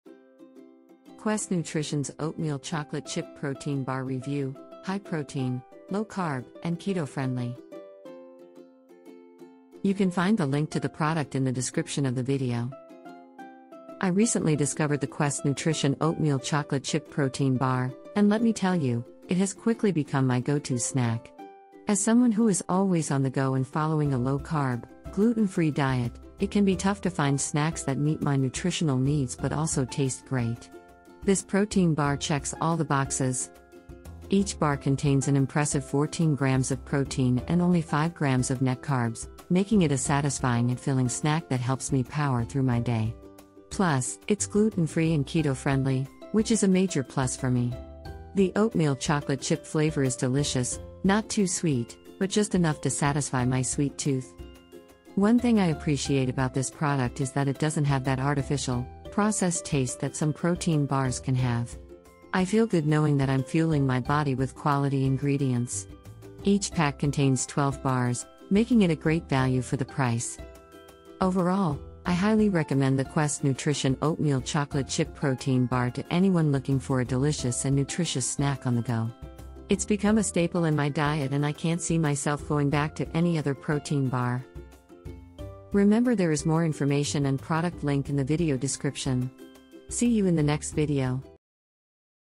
Quest Nutrition's Oatmeal Chocolate Chip Protein Bar Review: High Protein, Low Carb, and Keto-Friend
Quest Nutrition Oatmeal Chocolate Chip Protein Bar, High Protein, Low Carb, Gluten Free, Keto Friendly, 12 Count

00:00 - Intro
00:12 - 1) Quest Nutrition Oatmeal Chocolate Chip Protein Bar, High...
01:24 - Outro